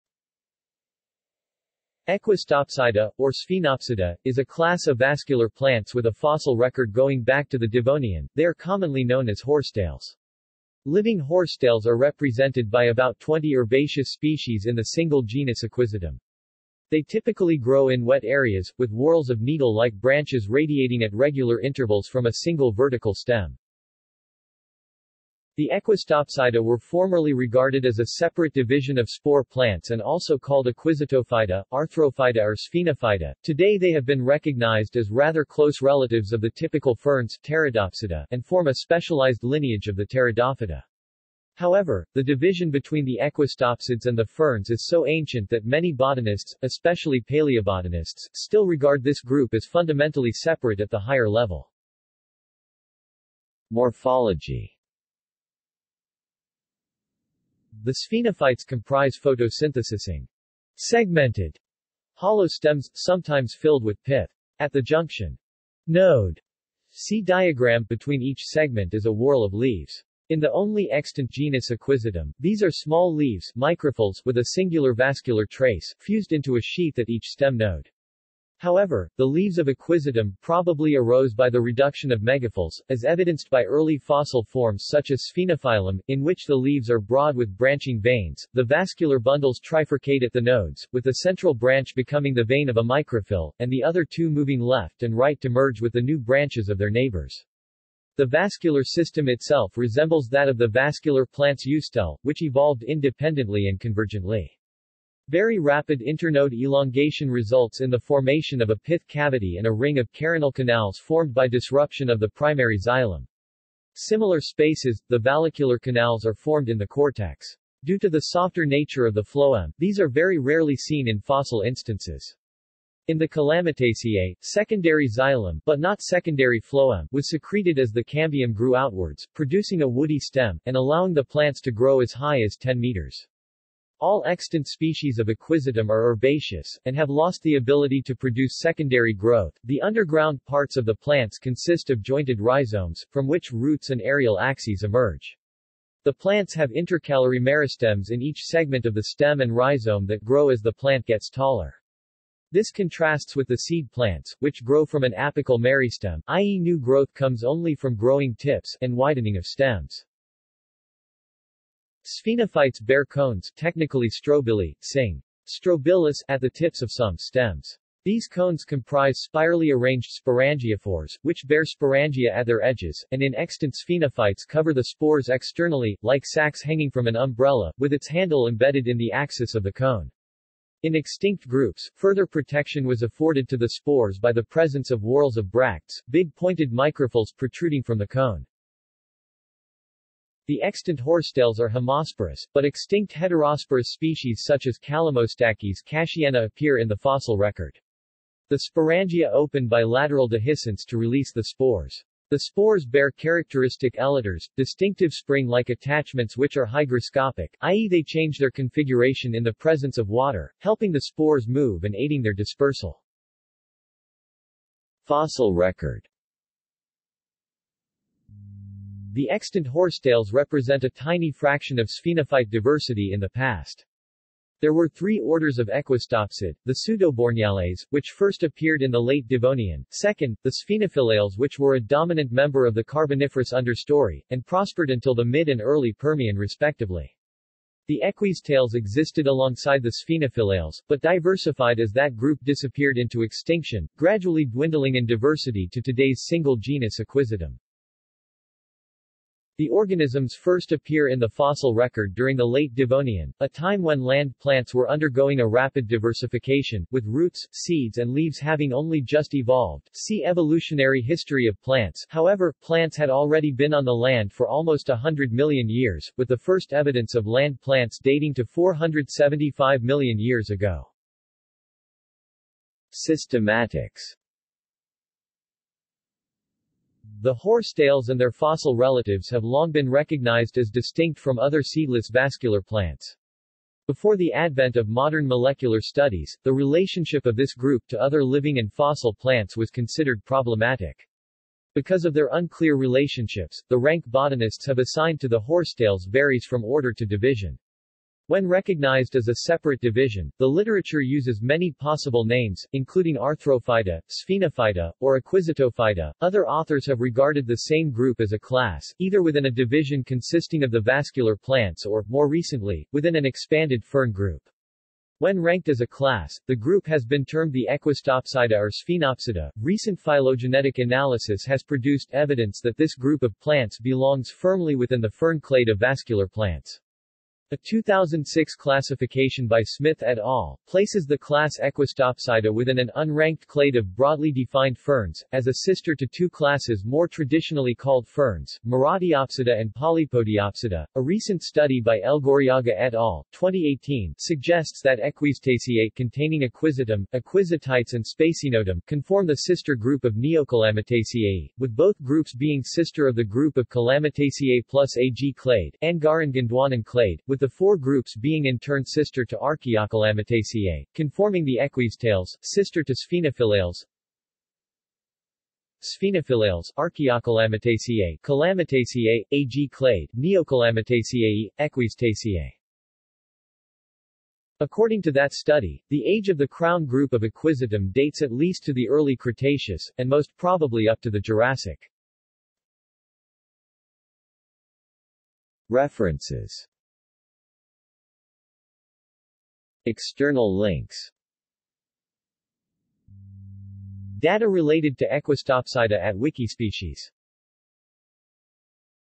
Equisetopsida | Wikipedia audio article
This is an audio version of the Wikipedia Article:
Equisetopsida

SUMMARY
Equisetopsida, or Sphenopsida, is a class of vascular plants with a fossil record going back to the Devonian.  They are commonly known as horsetails. Living horsetails are represented by about twenty herbaceous species in the single genus Equisetum. They typically grow in wet areas, with whorls of needle-like branches radiating at regular intervals from a single vertical stem.
The Equisetopsida were formerly regarded as a separate division of spore plants and also called Equisetophyta, Arthrophyta or Sphenophyta; today they have been recognized as rather close relatives of the typical ferns (Pteridopsida) and form a specialized lineage of the Pteridophyta. However, the division between the Equisetopsids and the ferns is so ancient that many botanists, especially paleobotanists, still regard this group as fundamentally separate at the higher level.